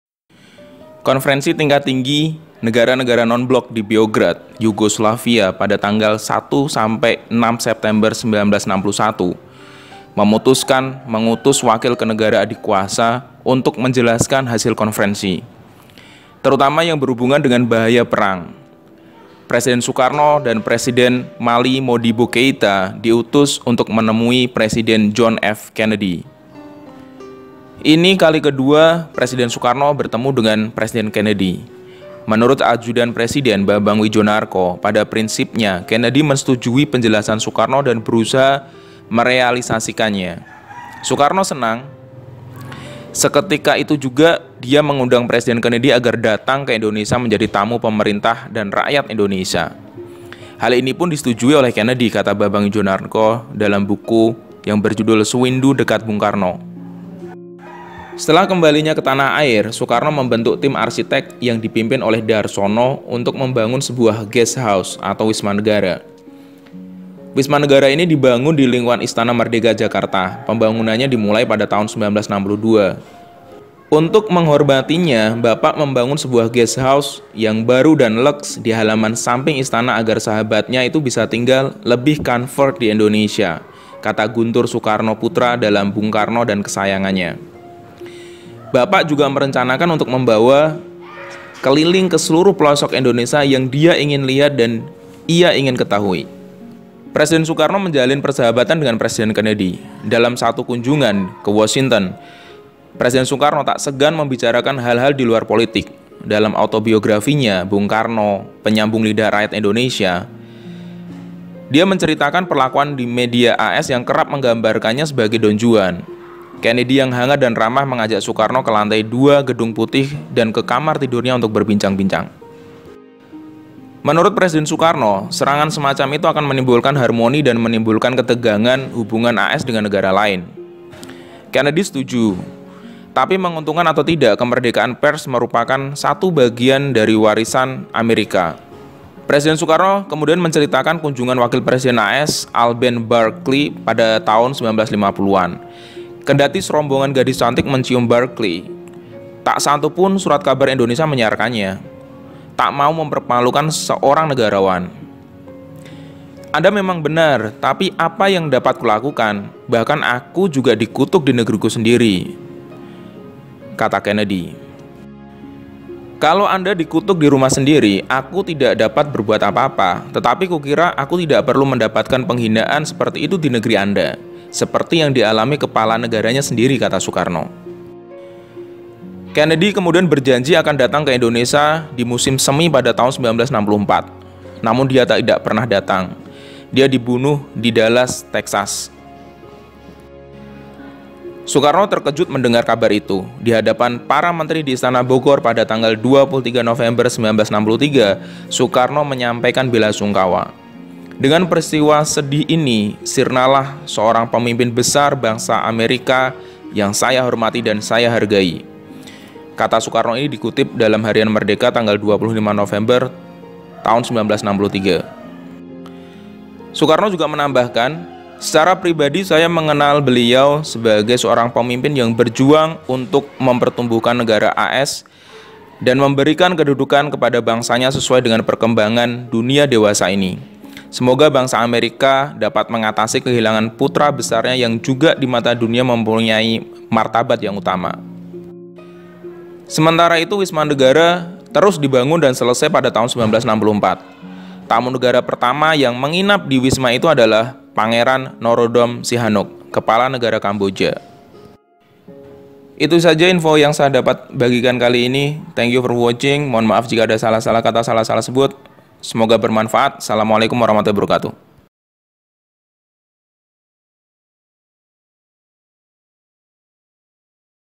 Sukarno Bangun Wisma Negara untuk Sahabatnya John F Kennedy
Ini kali kedua Sukarno bertemu Kennedy. Menurut ajudan presiden, Bambang Widjanarko, pada prinsipnya Kennedy menyetujui penjelasan Sukarno dan berusaha merealisasikannya. Sukarno senang. “Seketika itu juga dia mengundang Presiden Kennedy agar datang ke Indonesia menjadi tamu pemerintah dan rakyat Indonesia. Hal ini pun disetujui Kennedy,” kata Bambang Widjanarko dalam Sewindu Dekat Bung Karno.
Sekembalinya ke tanah air, Sukarno membentuk tim arsitek yang dipimpin Darsono untuk membangun guest house (wisma negara) di lingkungan Istana Merdeka Jakarta. Pembangunannya dimulai pada 1962.